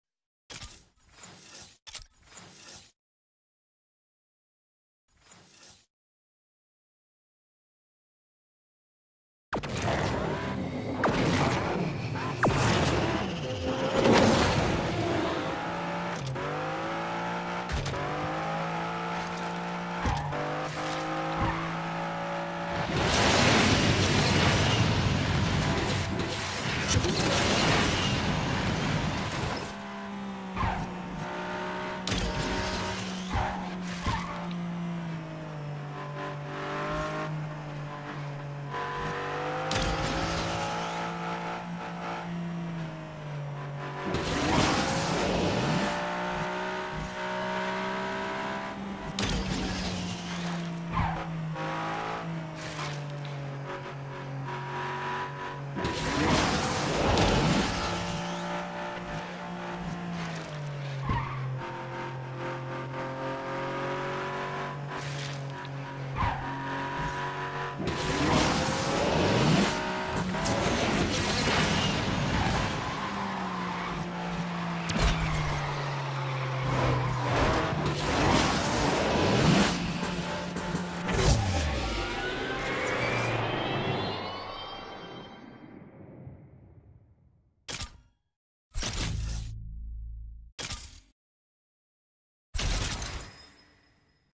1* with OC - 1:07:087 - Arc de Triomphe Paris - Round 1 - Ferrari Daytona SP3 Grand Prix
[[ bad lap ]]

My submission run for the Round 1 of the Ferrari Daytona SP3 grand prix
Final rounds from R1 to R6 after qualifiers
Keyboard manual drive player